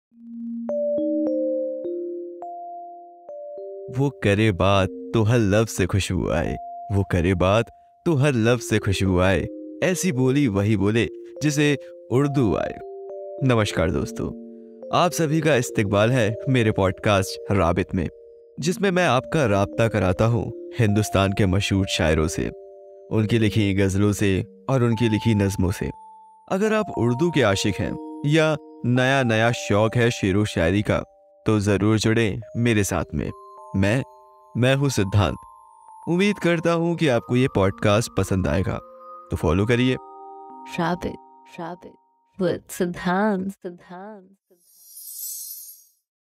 Raabit Trailer| Siddhant| World's Great Literature
Welcome to the Raabit a Urdu Poetry Podcast, where we explore the rich world of Urdu poetry and its fascinating history.

Join us as we delve into the works of famous poets like MirzaGhalib, AllamaIqbal, FaizAhmedFaiz, and many more. Each episode features a new poem, read in both Urdu and English translation, followed by a discussion of its themes, imagery, and cultural significance.

Whether you're a lifelong fan of Urdu poetry or a newcomer to the genre, this podcast offers a window into a world of beauty and complexity. So sit back, relax, and let the power of Urdu poetry transport you to another time and place. My Instagram handle is @kahaaniwala_ladka